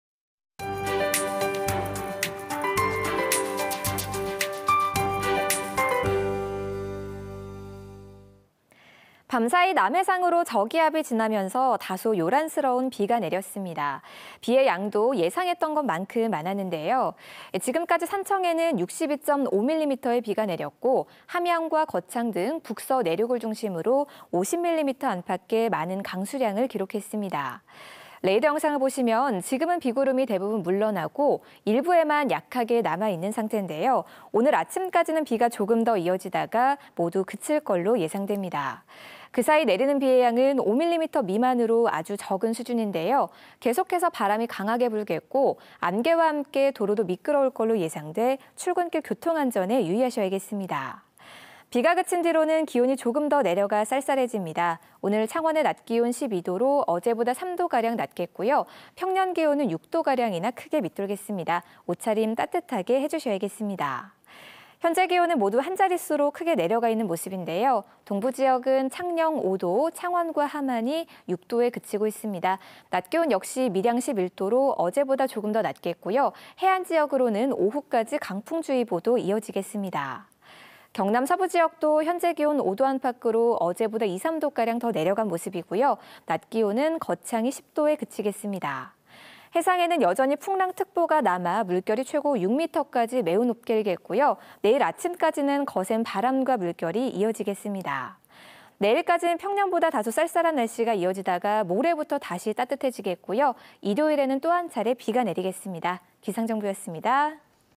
[경남의 날씨] 2019.4.10.(수) 기상 정보
밤사이
남해상으로
저기압이
지나면서
우리 지역에
다소 요란스러운
비가 내렸습니다.
비의 양도
예상했던 것만큼
많았는데요.
지금까지
mm의 강수량을
기록했습니다.

레이더 영상을
보시면
지금은
비구름이 거의
물러나고,
일부에만
약하게 남아 있는
상태인데요.
오늘 아침까지는
비를 조금 더
뿌리다가
모두 물러날 걸로
예상됩니다.

그 사이
내리는 비의 양은
5mm 미만으로
아주 적겠는데요.
오후까지는
강한 바람이
계속되고,
안개와 함께
도로가
다소 미끄러울 걸로
예상돼
주의하셔야겠습니다.

비가 그친 뒤로는
기온이 조금 더
내려가 쌀쌀해집니다.
오늘 창원의
낮 기온은 12도로
어제보다 3도가량
낮겠고요.
평년 기온은
5도가량
크게 밑돌겠습니다.
옷차림을
따뜻하게
하고 나오셔야겠습니다.

해상에도
여전히 풍랑특보가
이어지며
바람이 강하게
불고 있고요.
내일 아침까지는
거센 바람과 물결이
지속되겠습니다.

내일까지는
평년보다
다소 쌀쌀한 날씨가
이어지다가
모레부터
다시 따뜻해지겠고요.
일요일에는
또 한 차례
비가 내리겠습니다.